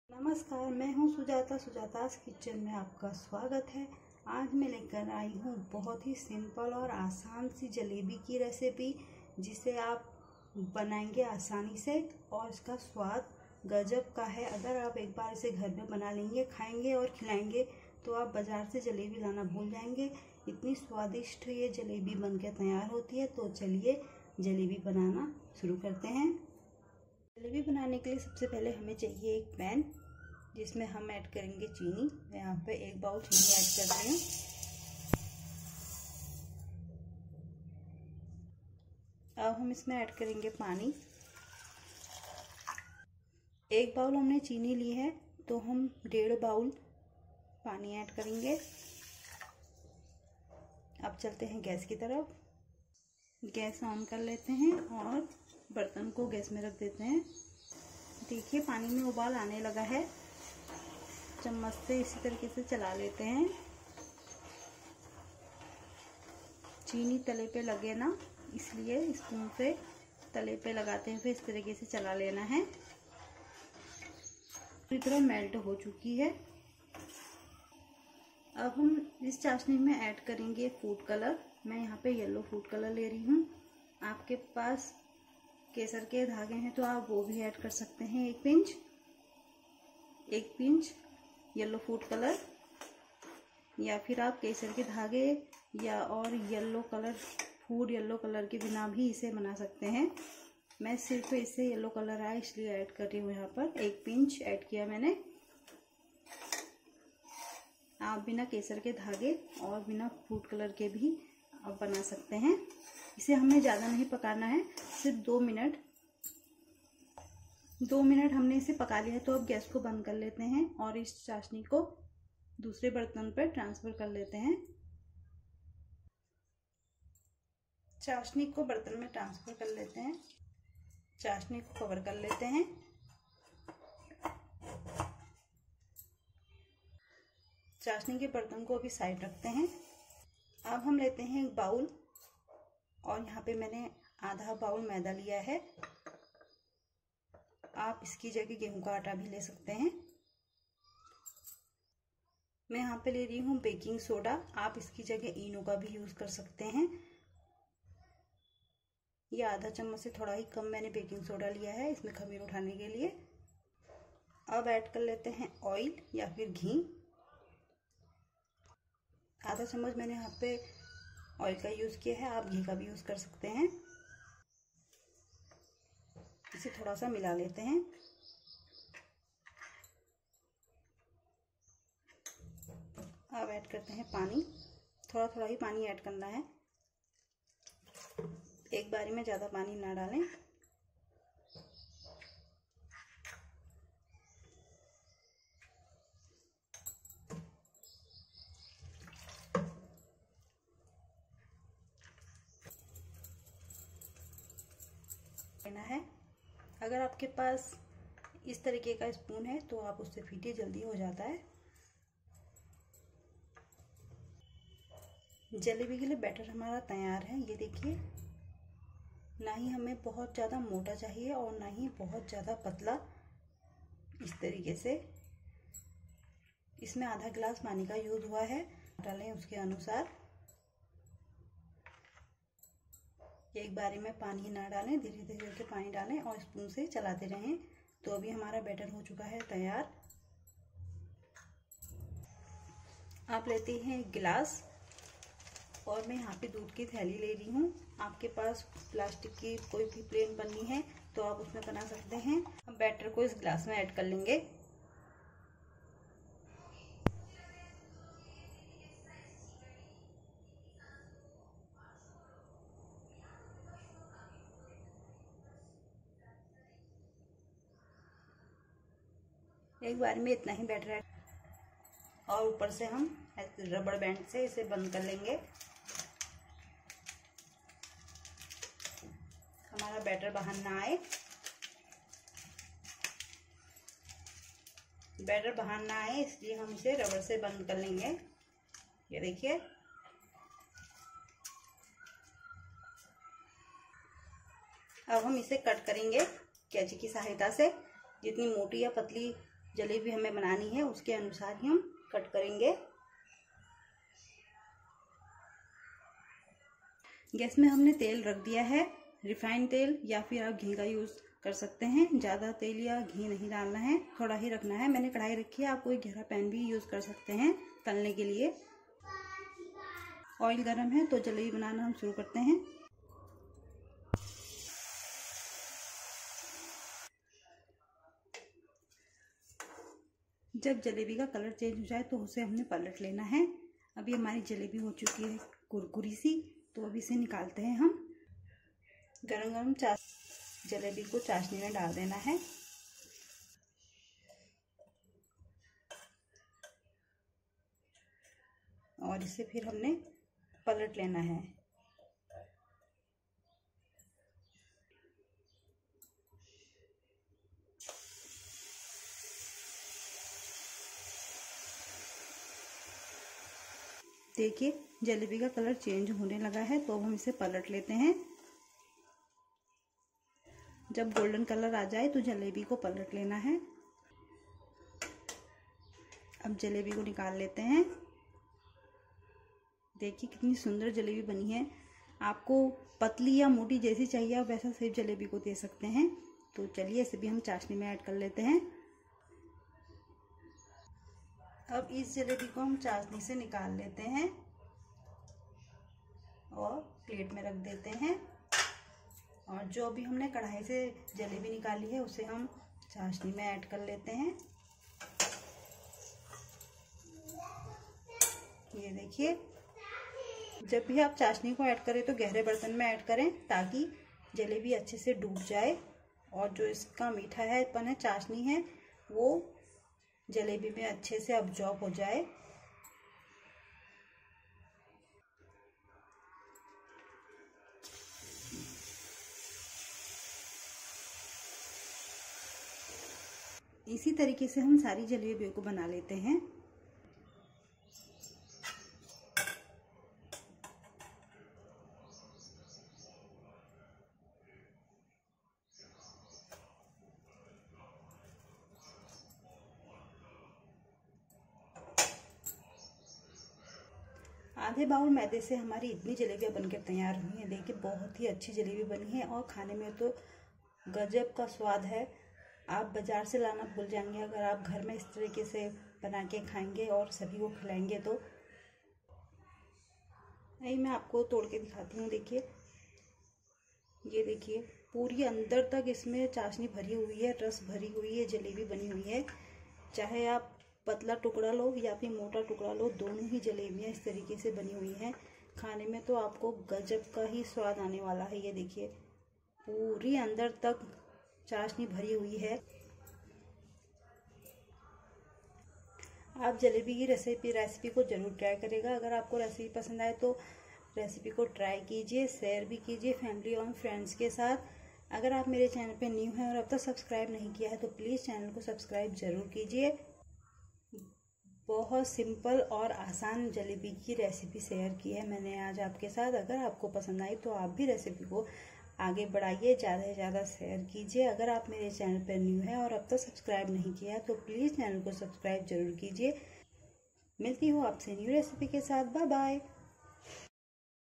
जलेबी झटपट बनाए बाजार जैसा स्वाद घर में ही | Jalebi recipe | जलेबी बनाने की विधि | sujata 's kitchen
hlo frends welcome beck to our channel
dosto jb bajar jaisa suwad ghar par mil jayega to har koi yahi banaega ।
Homemad jalebi recipe bohat he asan hai aap recipe ko pura dekhe or recipe pasand aye to make sure try it